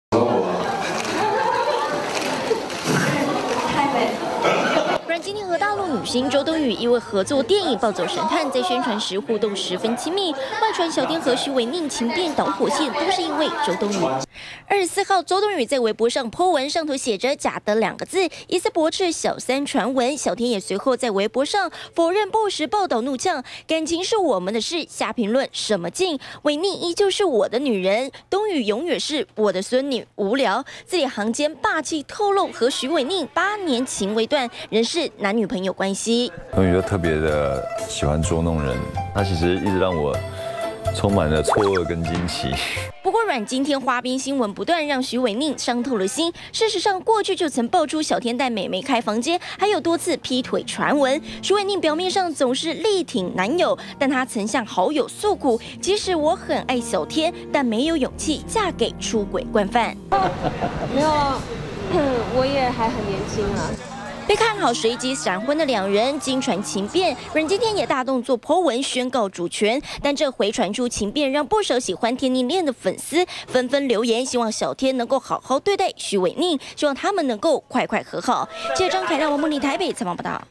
20150325中天新聞 回嗆情變！ 阮經天：瑋甯依然是我女人
阮經天和許瑋甯，驚爆8年情變目前分居中，外傳是因為小天太花心，指他沾上大陸女星周冬雨，24號周冬雨在微博PO「假的」兩字，不願被利用而駁斥傳聞，阮經天也否認不實報導怒嗆，感情是我們的事，瑋甯依然是我女人，他強調和許瑋甯，兩人在處於冷靜期，駁斥分手及小三傳聞。